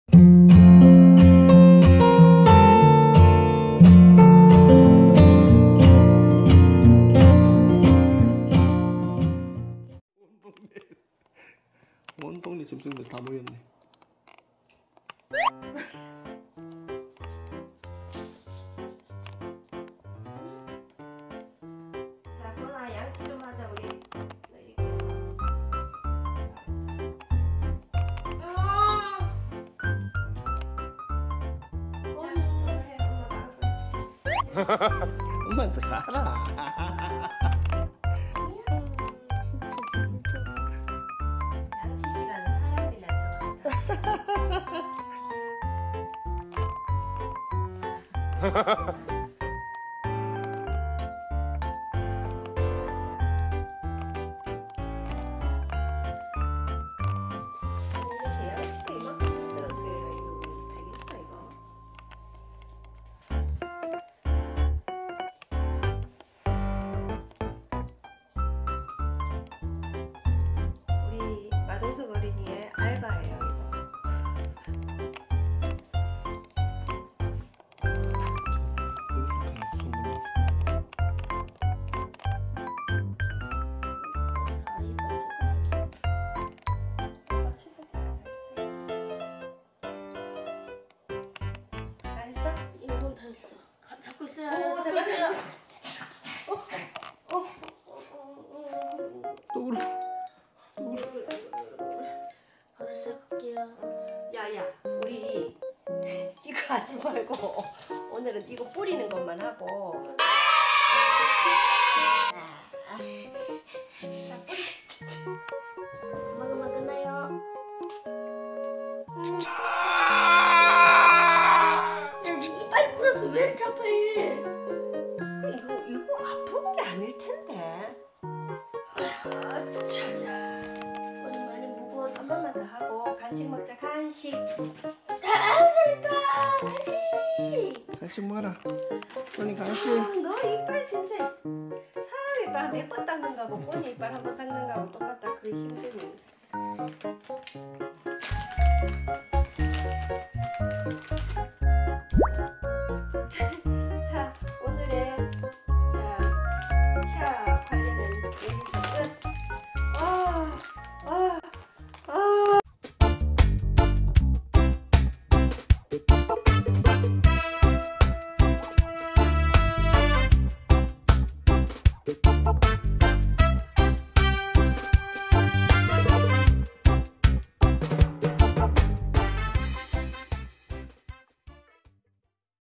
쫄보푸들꼬니. 강아지 양치질의 모든것😂🙈
토이푸들 강아지 꼬니.. 매일 마동석 오빠때문에 저런 표정을 지어요. 도대체 뭘 했길래 저런 표정을 짓는걸까요?ㅋㅋㅋㅋ 워크맨 처럼 마동석 오빠의 아르바이트 체험일기!

🍏[꼬니네 인스타그램] - 귀여운 꼬니 사진을 올리고 있어요~!